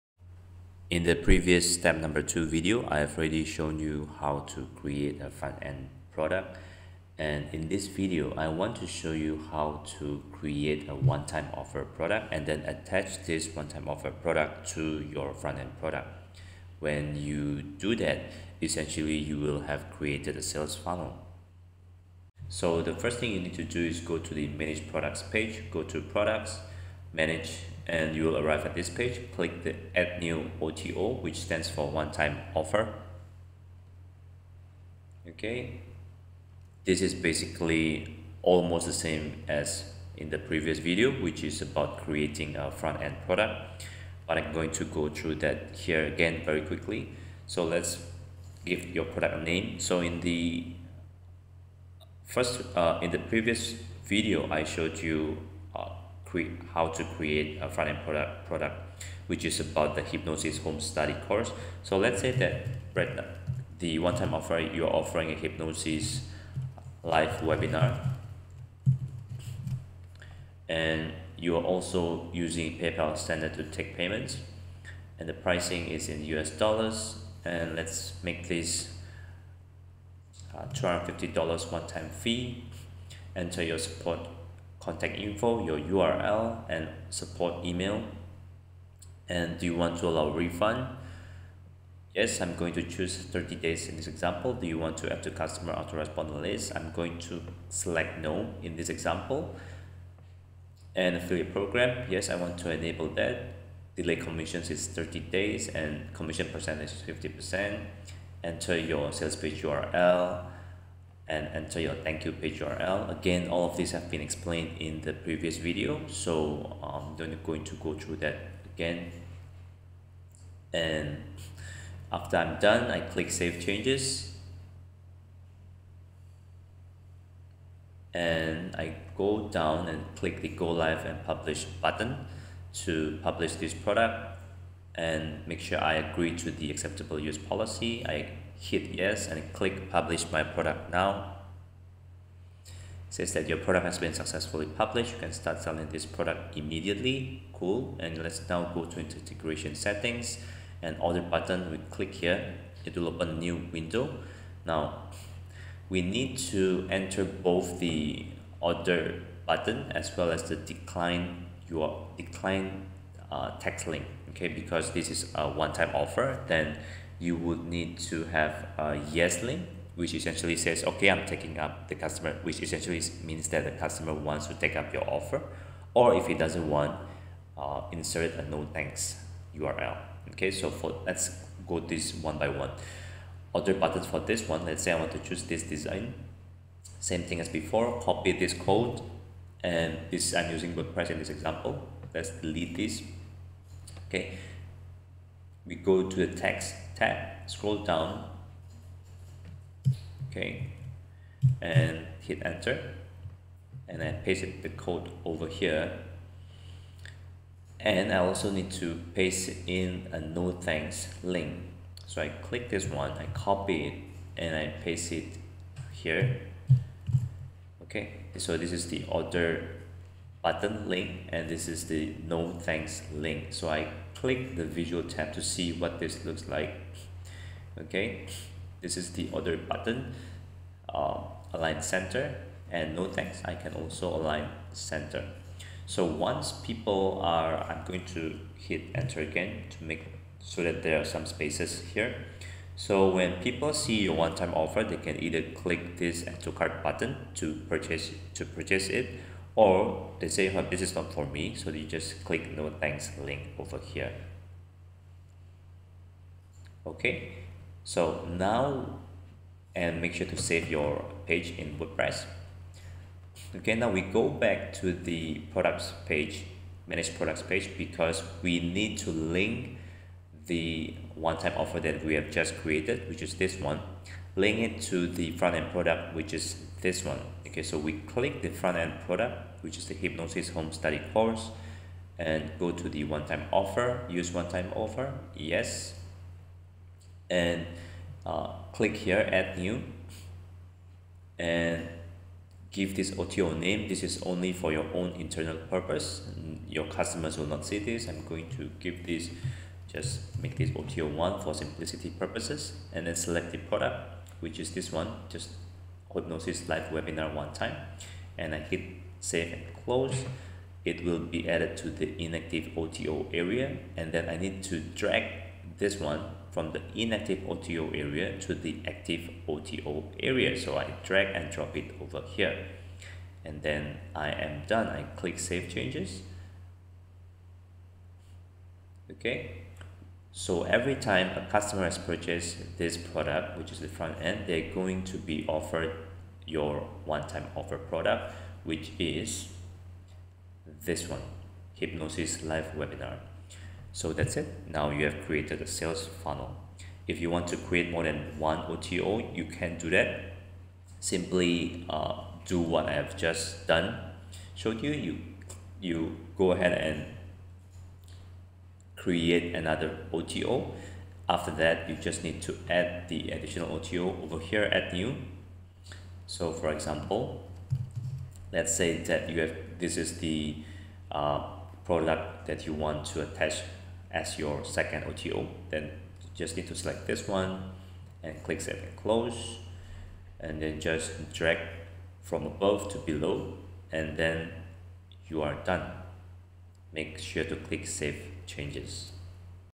Zaxaa Step 3 - Create A Sales Funnel
Zaxaa step-by-step tutorials for beginners to set up their first product for sale.